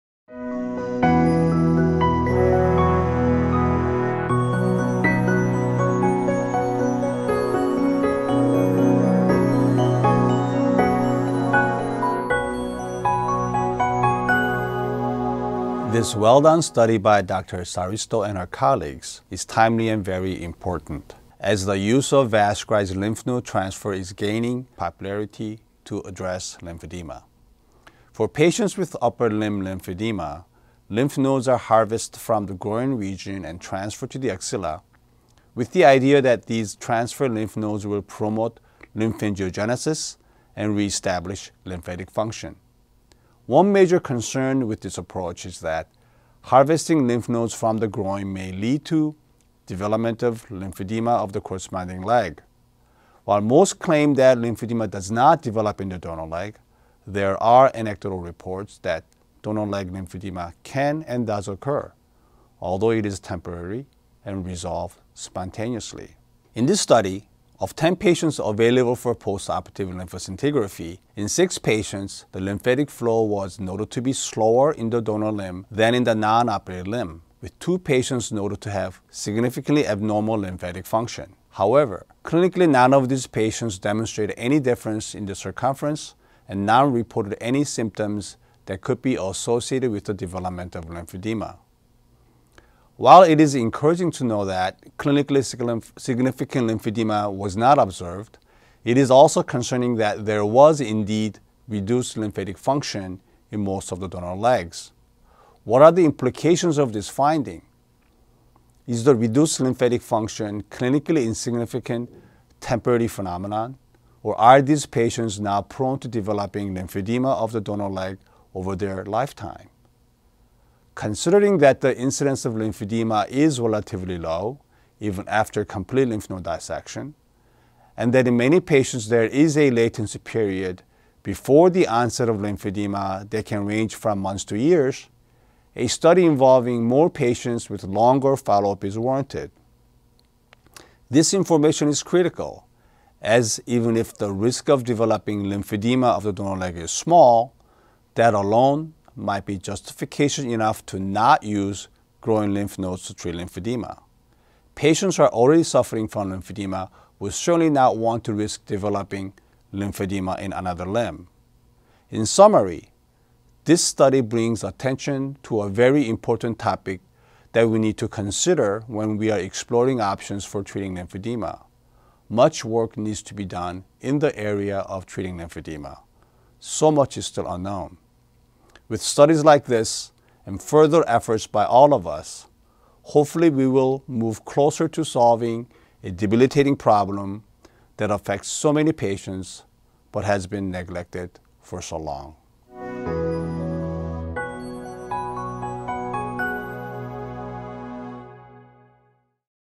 PRSJournal.com Video Discussion: David Chang on Donor-Site Lymphatic Function
In this video, David W. Chang, MD, FACS discusses the article "Donor-Site Lymphatic Function after Microvascular Lymph Node Transfer" by Viitanen et al. appearing in the December 2012 issue of Plastic and Reconstructive Surgery, Volume 130, Issue 6.